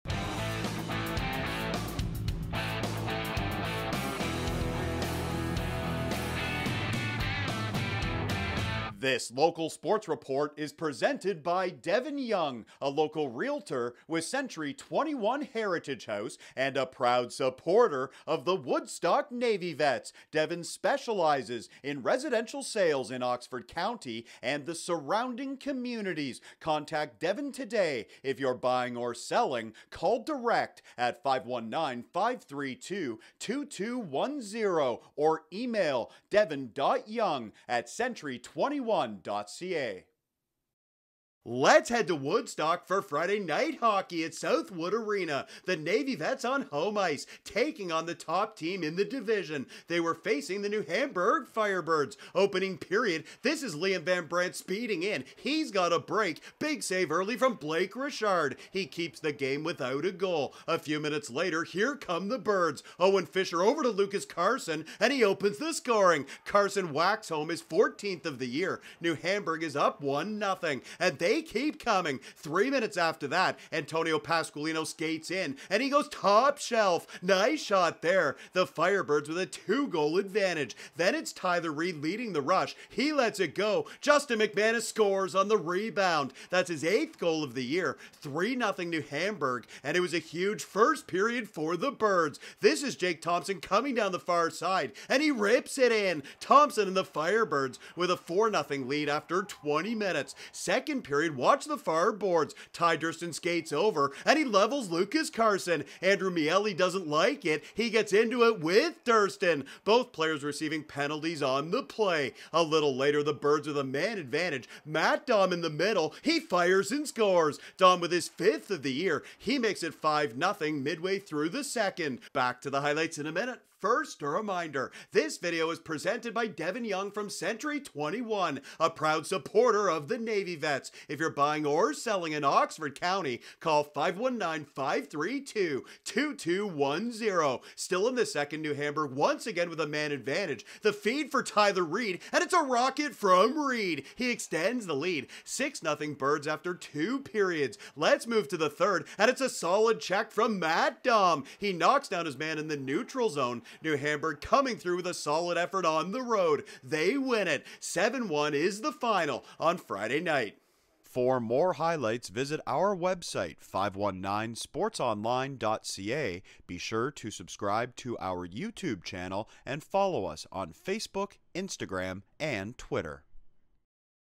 PJHL - New Hamburg Firebirds vs Woodstock Navy Vets
Highlights from Friday December 9th between the Firebirds and Navy Vets at Southwood Arena in Woodstock.

This video is presented by Devon Young from Century 21. Call Devon if you're buying or selling in Oxford County.